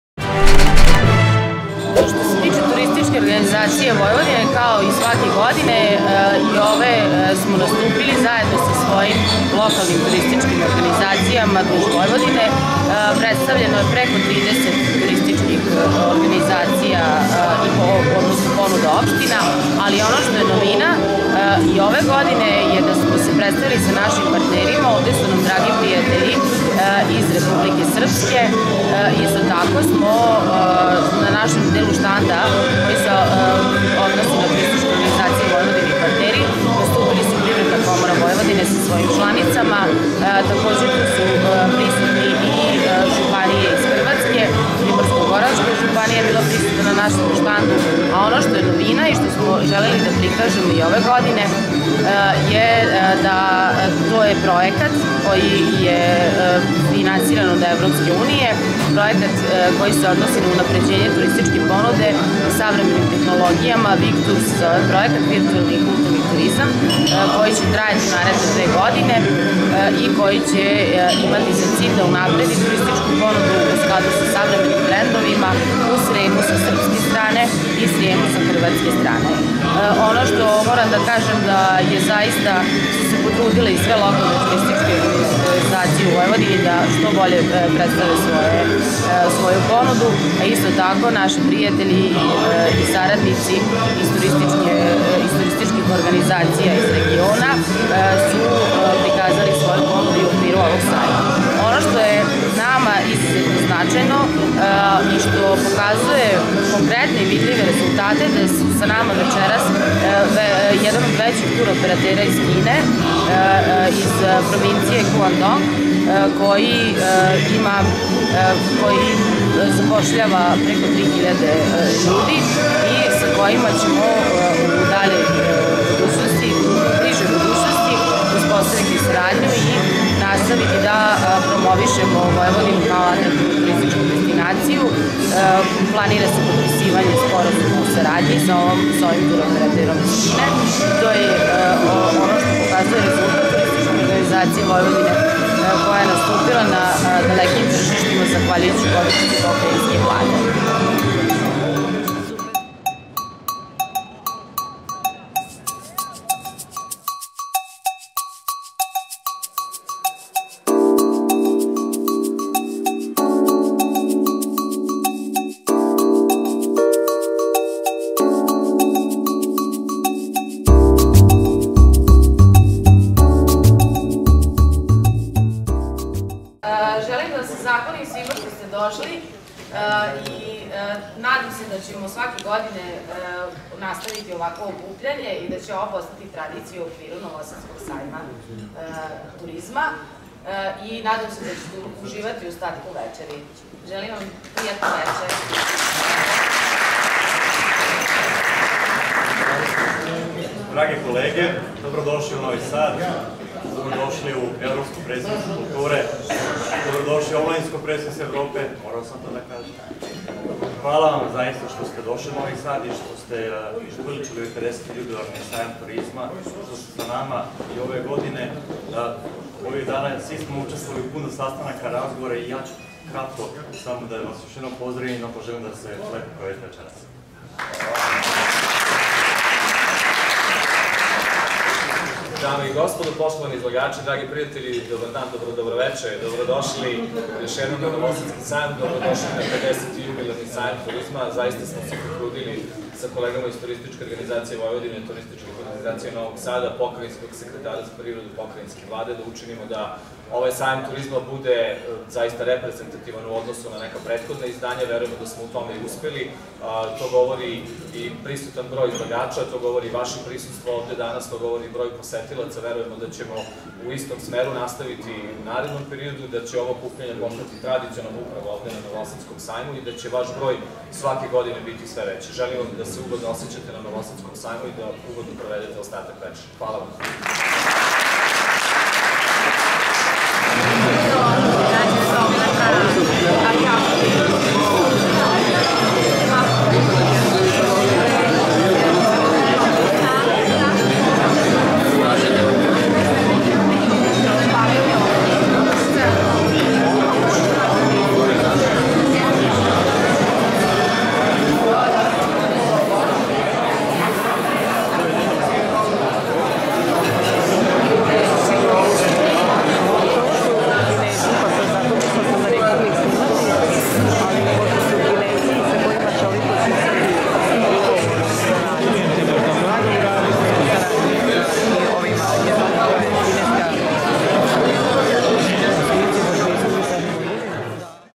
Turizam Vojvodine na Novosadskom sajmu turizma, od 5. do 7.10.2017.
Turisticka organizacija Vojvodine i ove godine je okupila više od 30 opštinskih turistickih organizacija, svojim partnerima omogucila regionalno predstavljanje na ovom sajmu i organizovala još nekoliko aktivnosti.
Nakon celodnevnog rada na štandovima turisticki poslenici su se okupili na koktelu koji je, kao domacin, organizovala Turisticka organizacija Vojvodine.